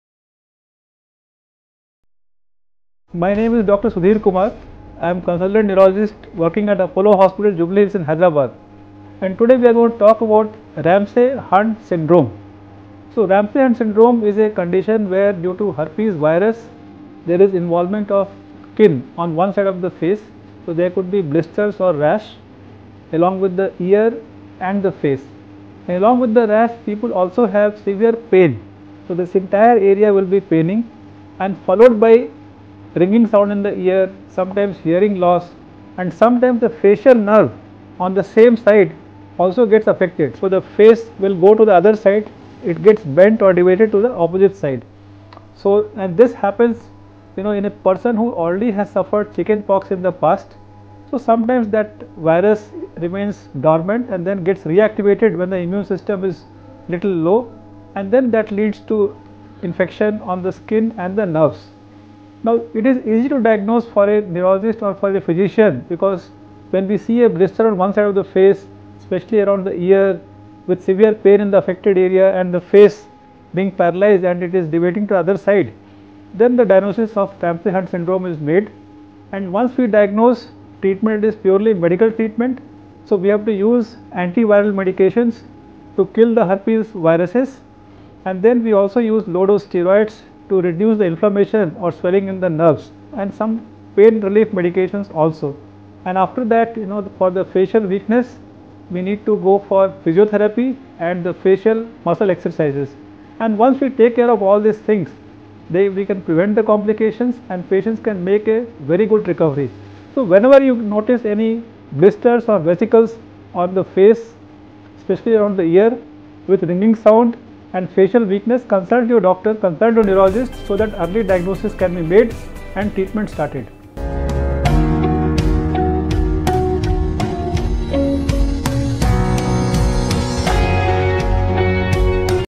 A Brief on Ramsay Hunt Syndrome | Dr. Sudhir Kumar | Apollo Hospital
Ramsay Hunt syndrome (RHS) is a rare neurological disorder characterised by paralysis of the facial nerve (facial palsy) and a rash affecting the ear or mouth. Listen to Dr Sudhir Kumar, Consultant Neurologist at Apollo Hospitals, Jubilee Hills, Hyderabad explain in detail about Ramsay Hunt Syndrome.